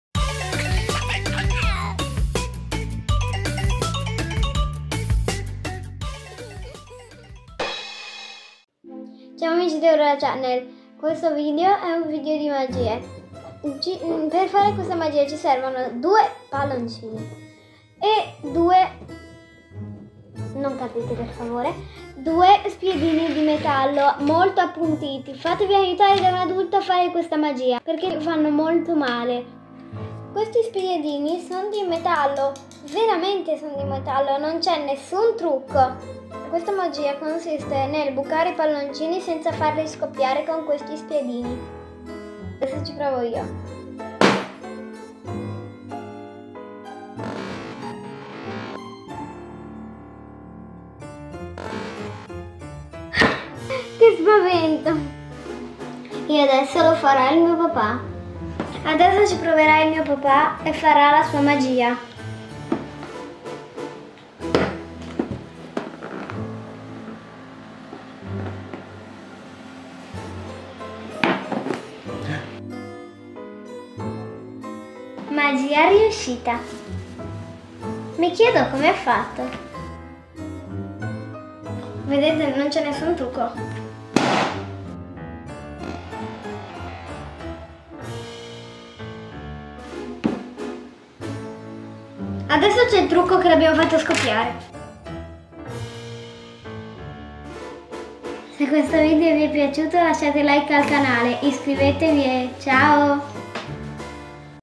MAGIA INCREDIBILE CON UN PALLONCINO - (AURORA CHANNEL) LE MAGIE DI AURORA
Ciao amici, oggi vi mostrerò un trucco di magia davvero sorprendente con un semplice palloncino. Sono sicura che questo incredibile trucco lascerà tutti a bocca aperta.

Non vedo l'ora di condividere con voi questa incredibile esperienza di magia con il palloncino.